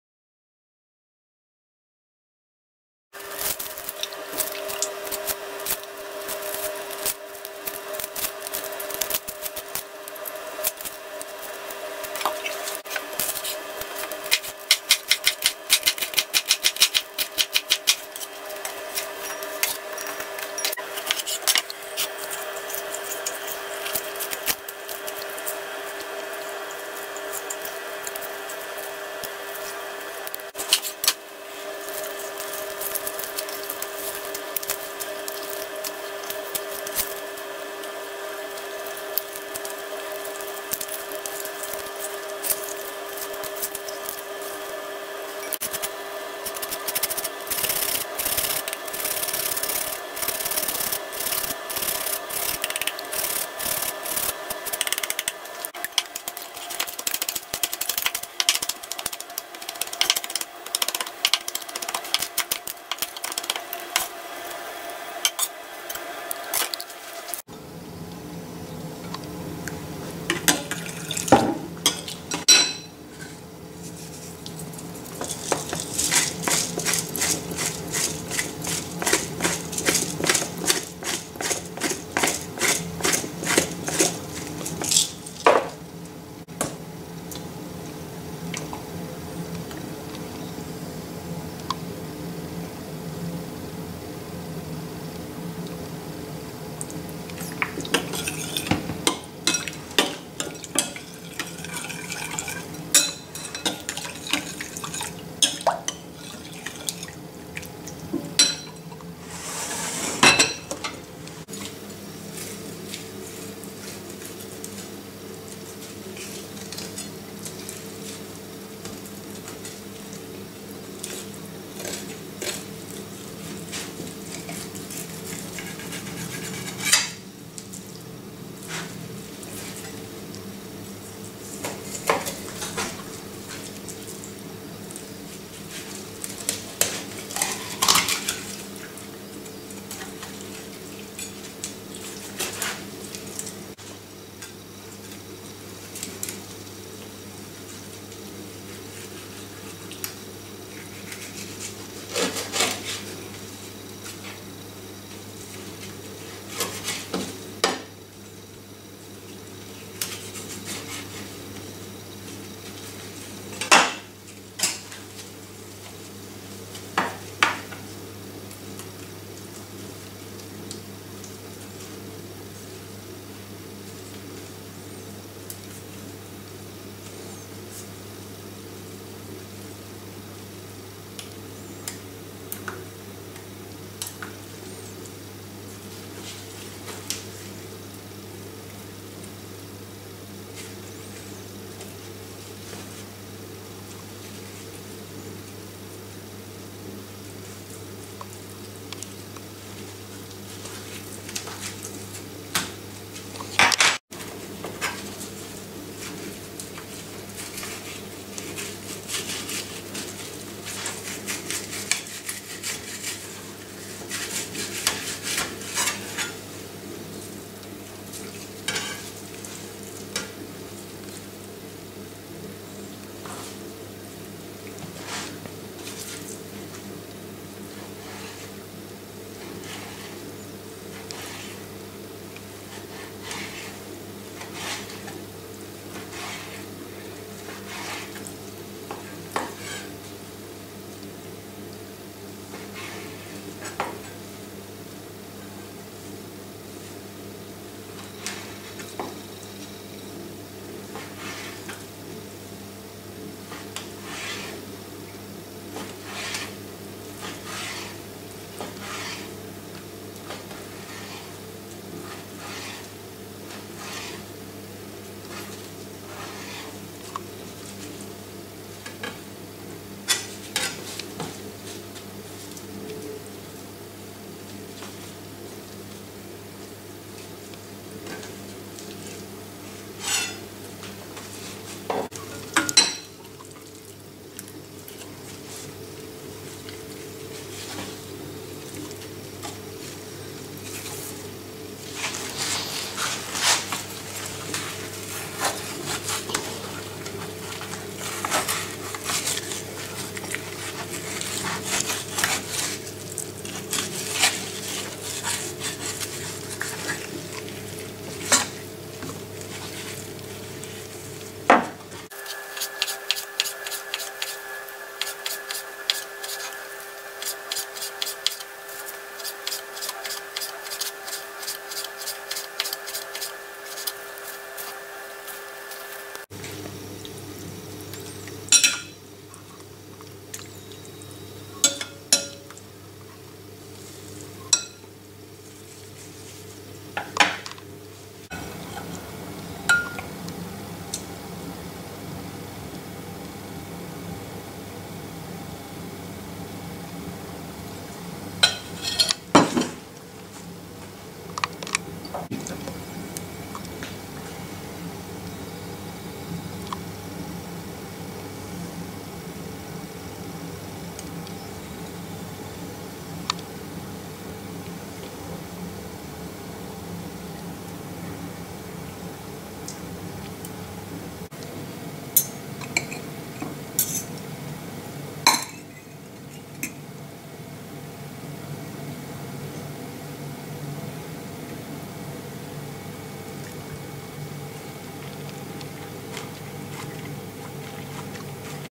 Севіче із сібаса - Sea bass ceviche
Севіче - латиноамериканська страва з риби, смачна та легка у приготуванні.

Ceviche is a traditional Latin American fish dish, delicious and easy to prepare. In this case I added some salsa made of mango and oranges.